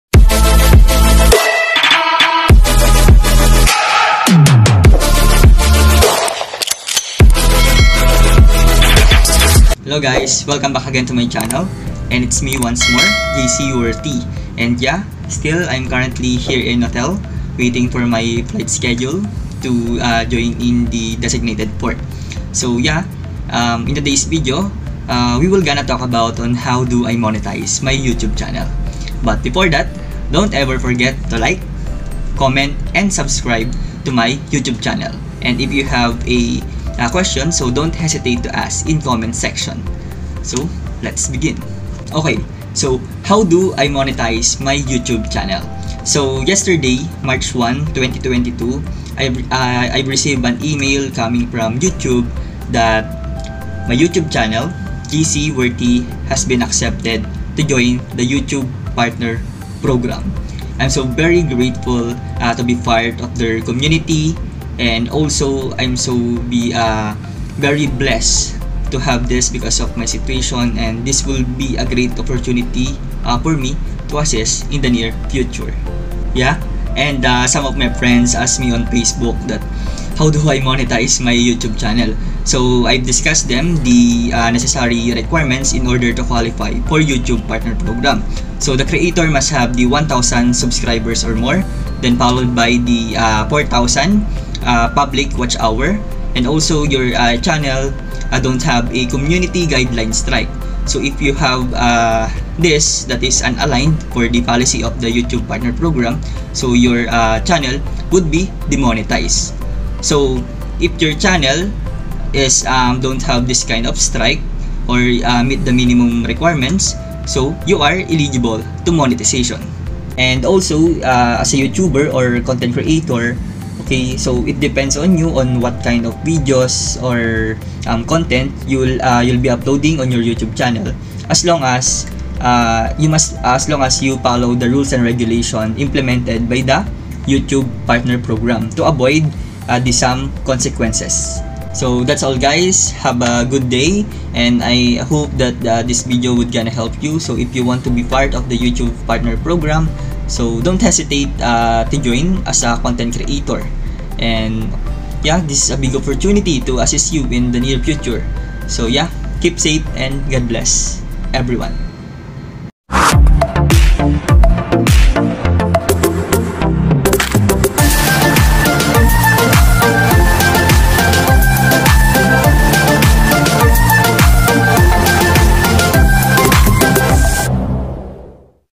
Monetization Approve || How do I monetize my Youtube Channel || Tips & Ideas.
Hello guys! Welcome back again to my channel it's me once more JSeaWorthy and for today's video were gonna talk about on How do I monetize my Youtube Channel and also what are the necessary requirements in order to qualify for Youtube Partner Program.